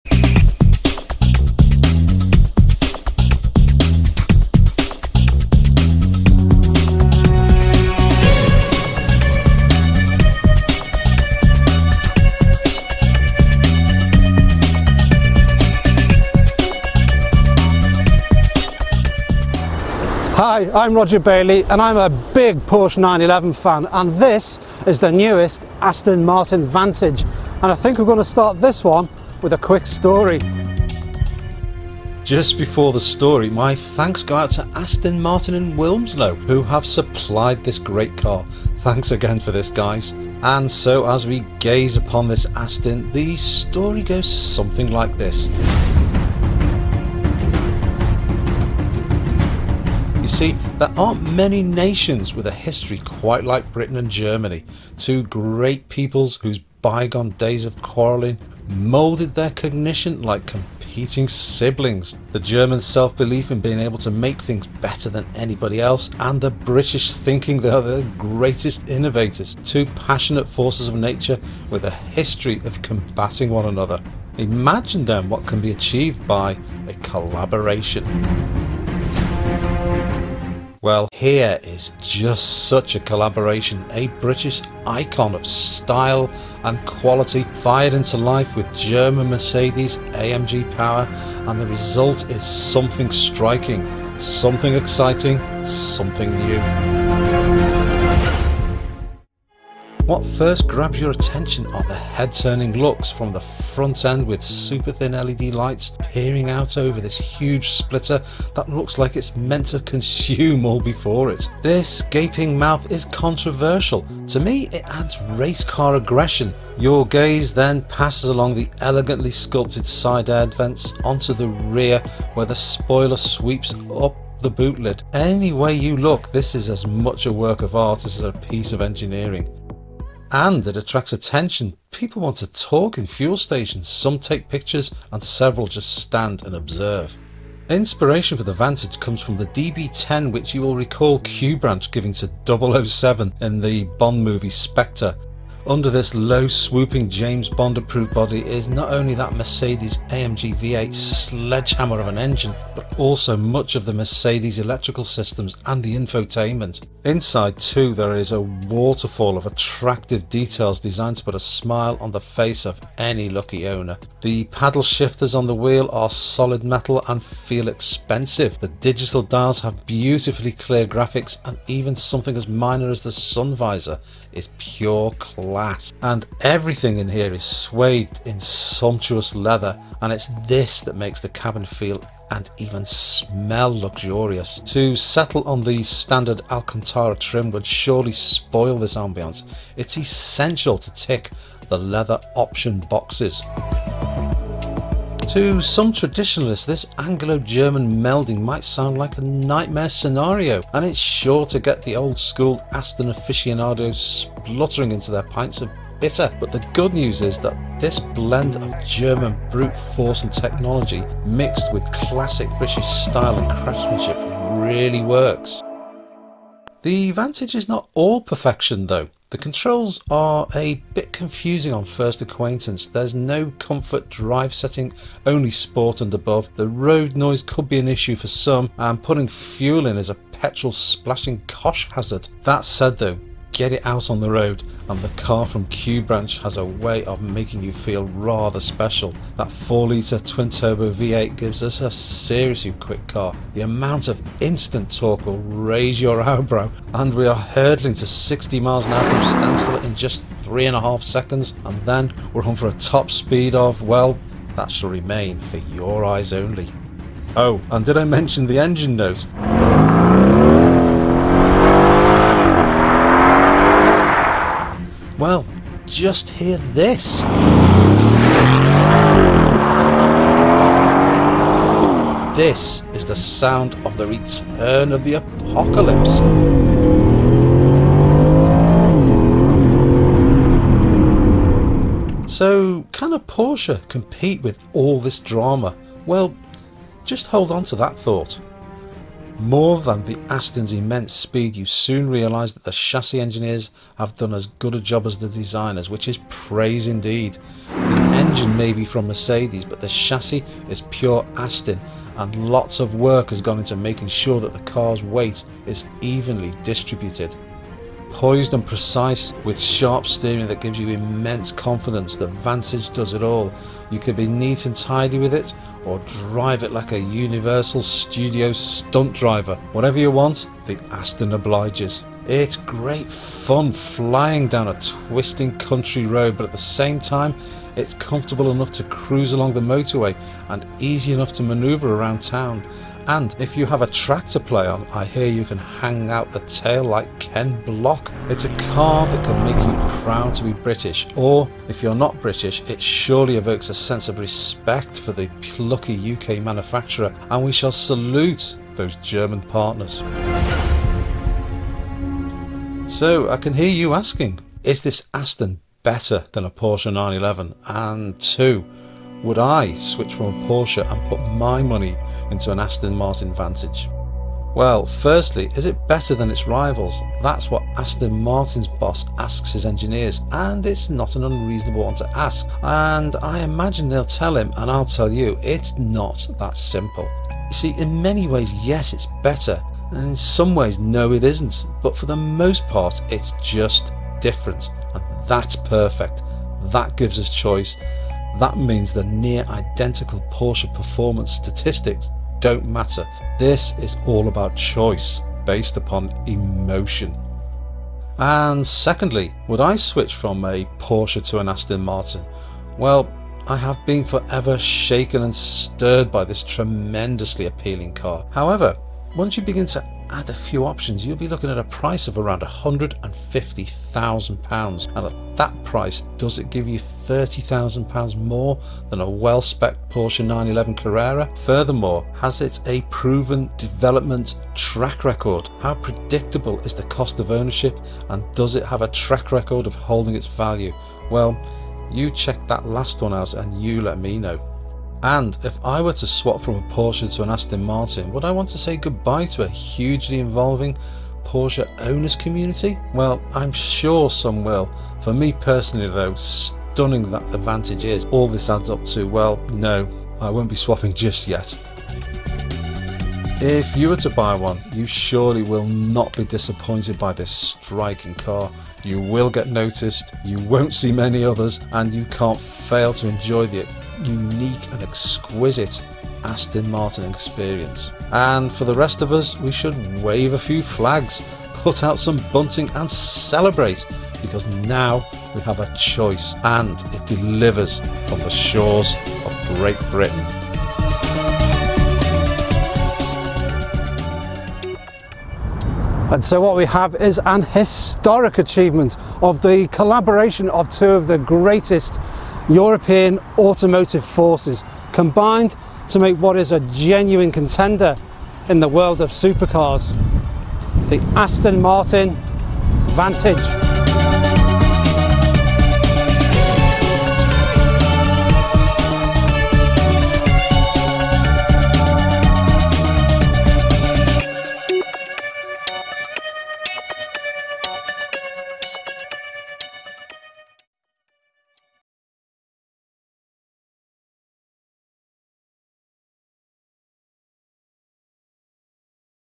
Aston Martin Vantage - A Porsche Enthusiasts Verdict ?
As a Porsche Enthusiast I had to try out one of the 911's competitors. The Aston Martin Vantage is special, it's seriously quick, has striking looks and you soon realise that the chassis engineers have done as good a job as the designers — which is praise indeed. The engine may be from Mercedes but the chassis is pure Aston.

But would I change from a Porsche ?  this video has a verdict.

Errors and Omissions Excepted
Use of drone conforming with CAA guidance
Footage obtained on a non commercial basis